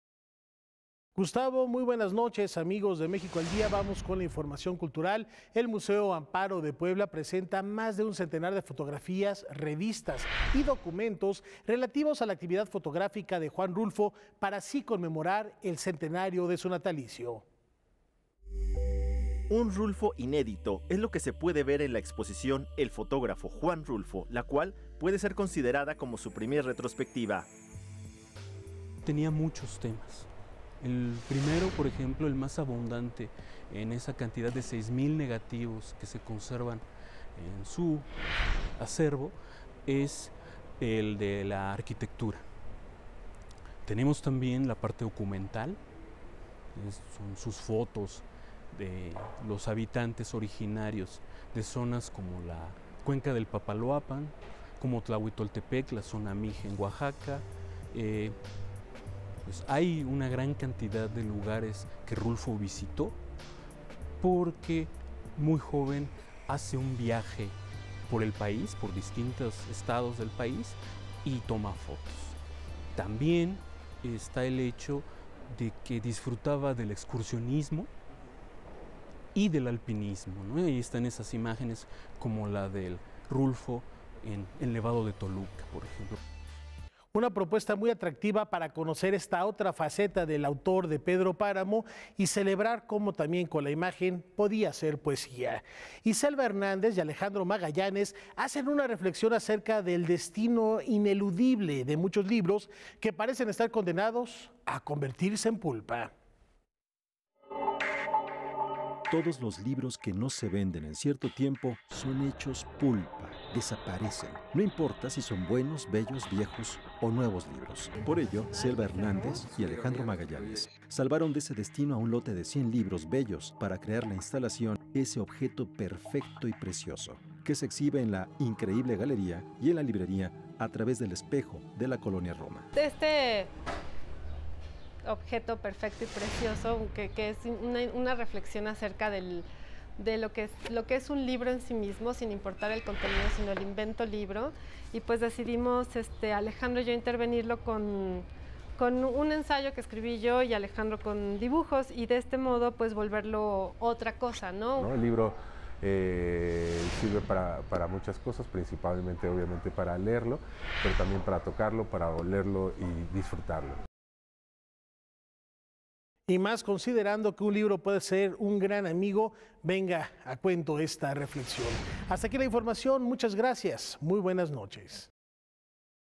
17/05/17 Cultura Canal
Un Rulfo inédito es lo que se puede observar en "El fotógrafo Juan Rulfo" en el Museo Amparo @MuseoAmparo
 Selva Hernández y Alejandro Magallanes instalan “Objeto perfecto y precioso” en La increíble librería y en la librería A través del espejo, de la Ciudad de México.